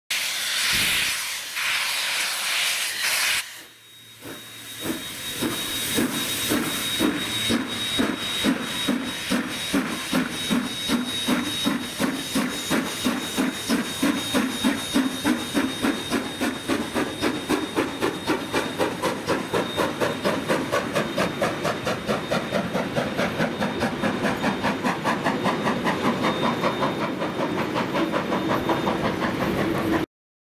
J A 1271 Leaves Te Awamutu today
J A 1271 Leaving Te Awamutu For  Te Kuiti Today.